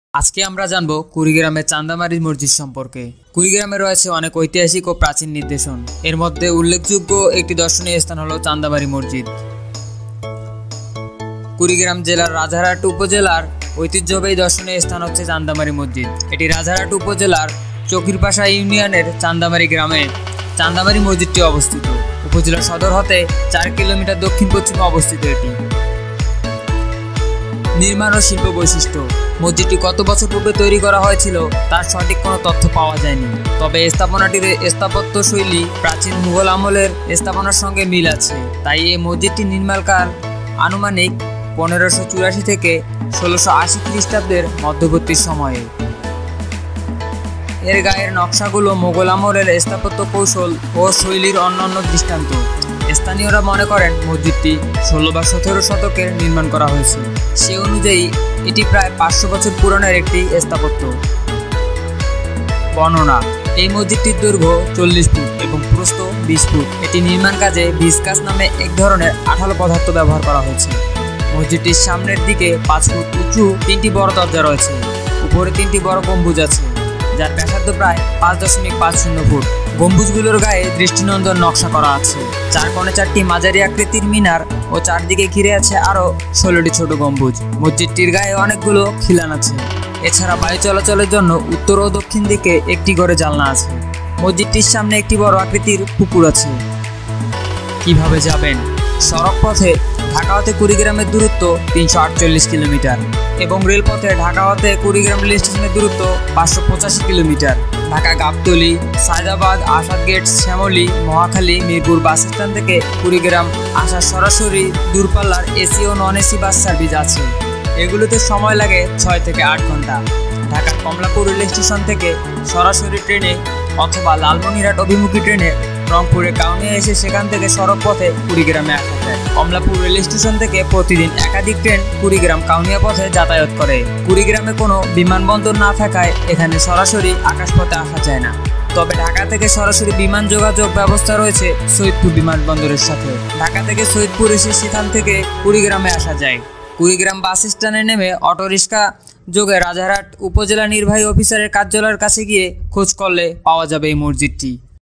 ৪শ বছর আগের চান্দামারী মসজিদ | Chandamari Mosque Rajarhat, Kurigram | Kurigram Historical Places
আজকে আমরা জানব কুড়িগ্রাম এর  চান্দামারি মসজিদ সম্পর্কেঃ

কুড়িগ্রামে রয়েছে অনেক ঐতিহাসিক ও প্রাচীন নিদর্শন।এদের মধ্যে উল্লেখযোগ্য  একটি দর্শনীয় স্থান হল  চান্দামারী মসজিদ।

কুড়িগ্রাম জেলার রাজারহাট উপজেলার একটি ঐতিহ্যবাহী দর্শনীয় স্থান হচ্ছে চান্দামারী মসজিদ । এটি রাজারহাট উপজেলার চকিরপাশার ইউনিয়নের চান্দামারী গ্রামে   “চান্দামারী মসজিদটি” অবস্থিত। উপজেলা সদর হতে ৪ কিঃমিঃ দক্ষিন পশ্চিমে অবস্থিত।

নির্মাণ ও শিল্প বৈশিষ্ট্যঃ
 মসজিদটি কতো বছর পুর্বে তৈরী করা হয়েছিল তার সঠিক কোন তথ্য পাওয়া জায়নি, তবে স্থাপনাটির স্থাপত্য শৈলী প্রাচীন মুঘল আমলের স্থাপনার সঙ্গে মিল আছে।এই মসজিদটির নির্মাণকাল আনুমানিক ১৫৮৪-১৬৮০ খ্রিটাব্দের মধ্যবর্তী সময়ে। এর গায়ের নকশা গুলো মোঘল আমলের স্থাপত্য কৌশল ও শৈলীর অনন্য দৃষ্টান্ত। স্থানীয়রা মনে করেন মসজিদটি ১৬ বা ১৭ শতকে নির্মান করা হয়েছিল। সে অনুযায়ী এটি প্রায় ৫০০ শত বছরের পুরাতন একটি স্থাপত্য।

বর্ণনা:
এই মসজিদটি দৈর্ঘ্যে ৪০ ফুট এবং প্রস্থে ২০ ফুট। এর নির্মাণকাজে ভিসকাস নামে এক ধরনের আঠালো পদার্থ ব্যবহার করা হয়েছে। মসজিদের সামনের দিকে পাঁচ ফুট উঁচু তিনটি বড় দরজা রয়েছে । ওপরে তিনটি বড় গম্বুজ আছে যার ব্যাসার্ধ প্রায় ৫.৫০ফুট। গম্বুজ গুলোর গায়ে দৃষ্টিনন্দন নকশা করা আছে। চার কোণায় চারটি মাঝারি আকৃতির মিনার ও চারদিকে ঘিরে আছে আরো ষোলটি ছোট গম্বুজ।মসজিদের গায়ে অনেকগুলো খিলান আছে। এ ছাড়া বায়ু চলাচলের জন্য উত্তর ও দক্ষিণ দিকে একটি করে জানালা আছে। মসজিদটির সামনে একটি বড় আকৃতির  পুকুর আছে

কিভাবে যাবেন
সড়ক পথে     ঢাকা হতে কুড়িগ্রামের দূরত্ব ৩৪৮ কিলোমিটার    এবং রেলপথে ঢাকা হতে কুড়িগ্রাম রেল স্টেশনের দূরত্ব ৫৮৫ কিলোমিটার। ঢাকার গাবতলী, সায়েদাবাদ, আসাদগেট, শ্যামলী, মহাখালী, মিরপুর বাস স্টেশন থেকে কুড়িগ্রামে আসার সরাসরি দুরপাল্লার এসি ও নন-এসি বাস সার্ভিস আছে;      এগুলোতে সময় লাগে ৬ হতে ৮.০০ ঘন্টা।  ঢাকার কমলাপুর রেল স্টেশন থেকে ট্রেনে সরাসরি, অথবা লালমনিরহাট অভিমুখী ট্রেনে রংপুরের কাউনিয়া এসে সেখান থেকে সড়ক পথে কুড়িগ্রাম আসা যায়। কমলাপুর রেল স্টেশন থেকে প্রতিদিন একাধিক ট্রেন কুড়িগ্রাম – কাউনিয়া পথে যাতায়ত করে।  কুড়িগ্রামে কোনো বিমানবন্দর না-থাকায় এখানে সরাসরি আকাশ পথে আসা যায় না, তবে ঢাকা থেকে সরাসরি বিমান যোগাযোগ ব্যবস্থা রয়েছে সৈয়দপুর বিমানবন্দরের সাথে; ঢাকা থেকে সৈয়দপুর এসে সেখান থেকে সড়কপথে কুড়িগ্রাম আসা যায়। কুড়িগ্রাম বাস স্ট্যান্ড এ নেমে অটো রিক্সা যোগে রাজারহাট উপজেলা নির্বাহী অফিসারের কার্যালয়ের কাছে গিয়ে খোঁজ করলে পাওয়া যাবে মসজিদটি ।